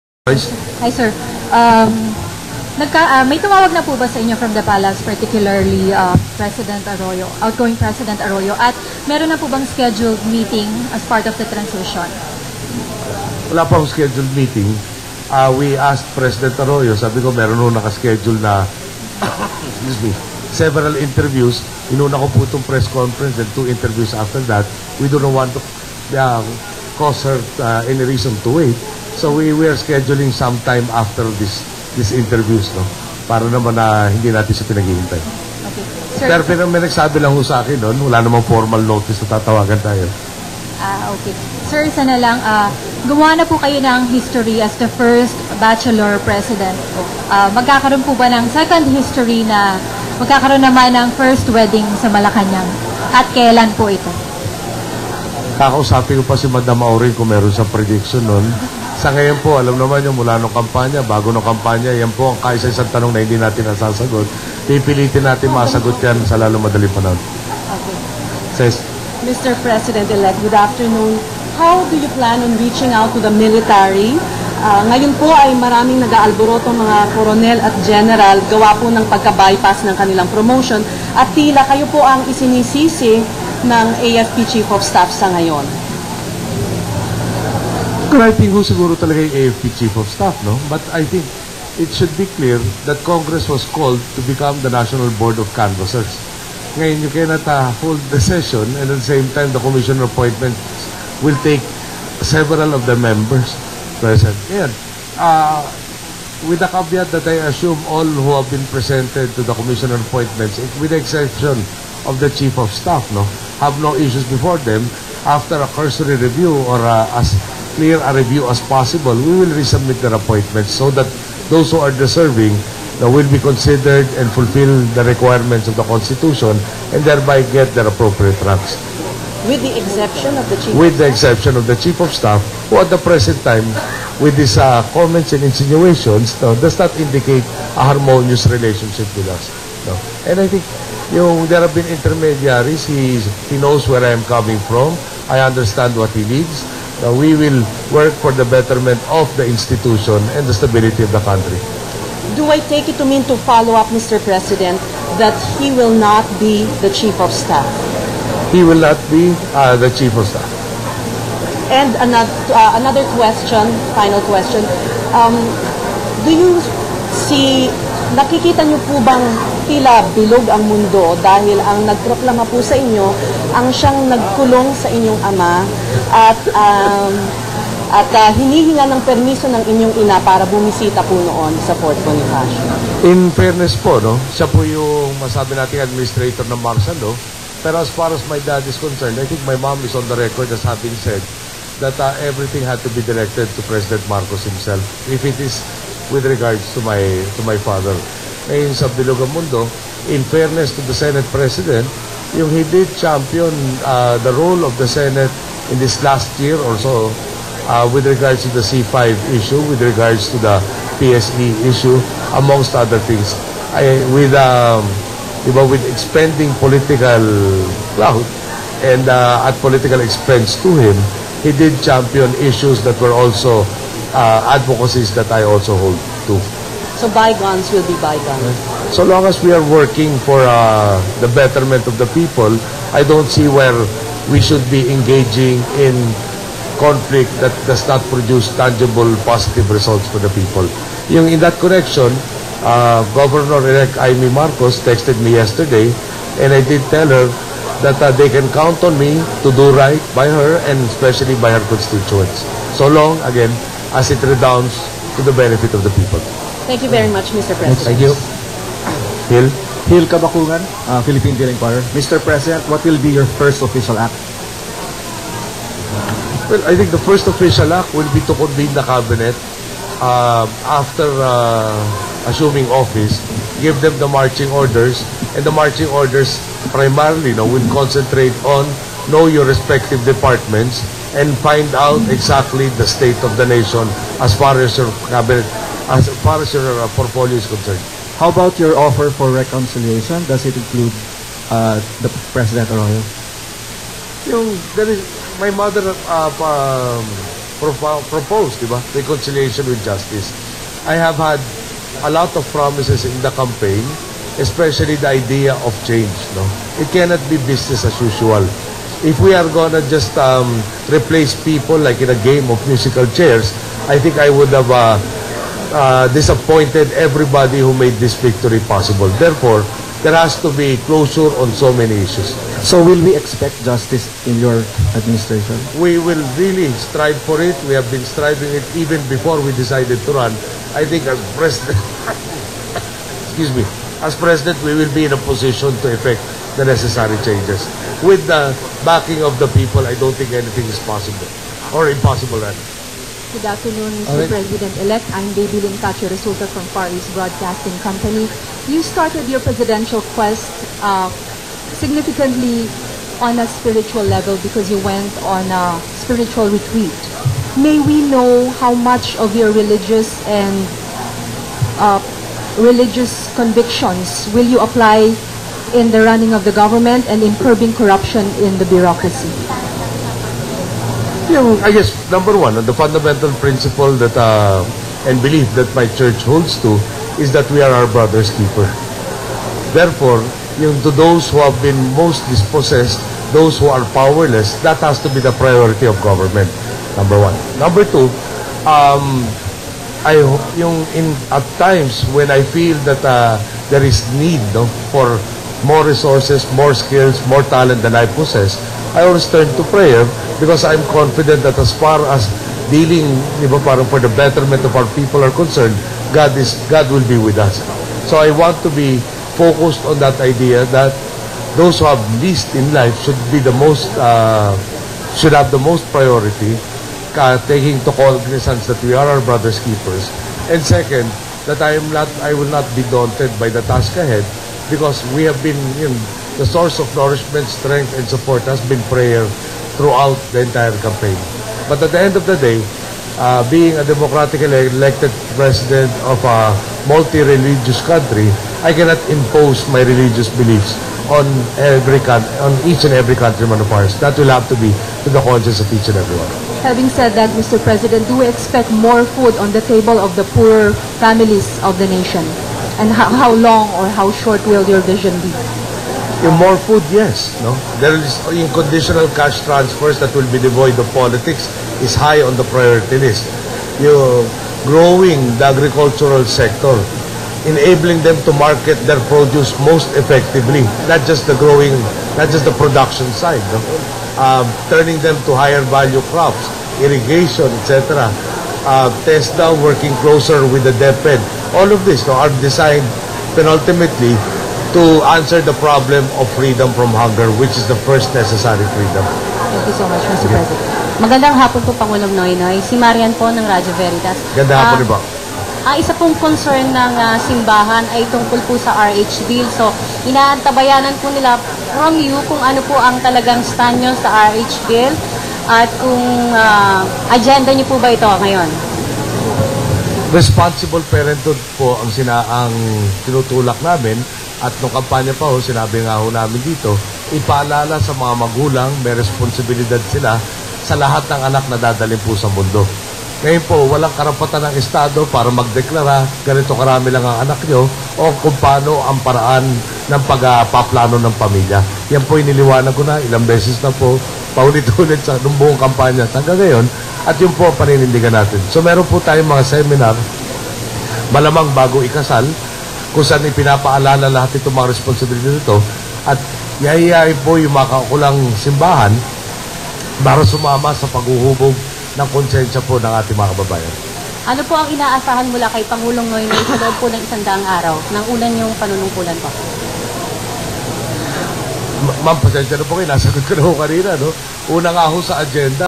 NoyNoy 1st press conference part2
NoyNoy 1st press conference
June 8, 2010
2010 Proclamation
Philippines President- Elect